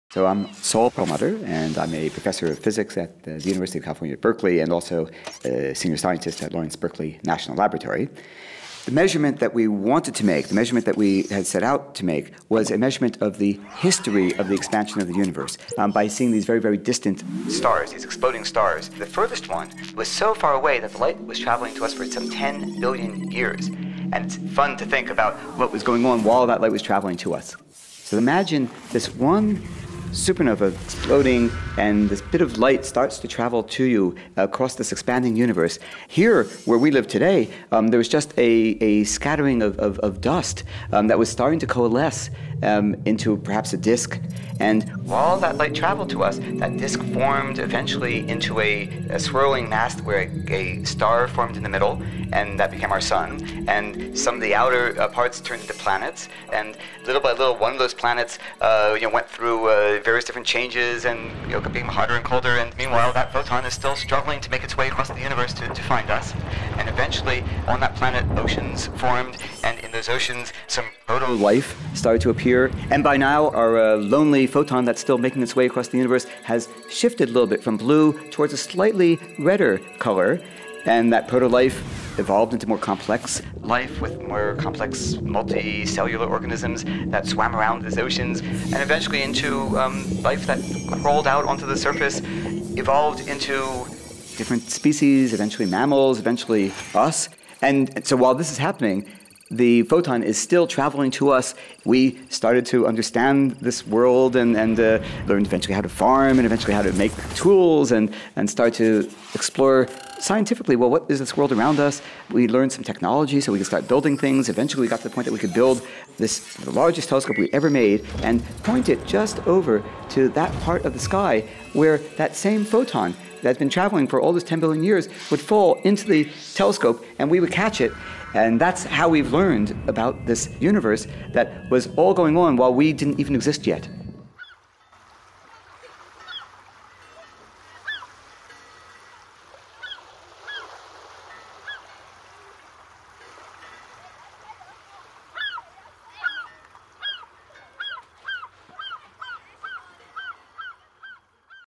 One Photon’s Journey: Saul Perlmutter
The third of four films for Nature about Nobel prize winners, recorded at Lindau 2015.

The story of the evolution of life on earth during one photon’s journey across the universe. Told by Saul Perlmutter who shared the 2011 Nobel Prize in physics for the discovery of the accelerating expansion of the universe.

Directed, illustrated, animated and generally powerfully powered up by Dog & Rabbit's powerhouse of a man, Fu (Monkey). Sound design by Nigel Manington.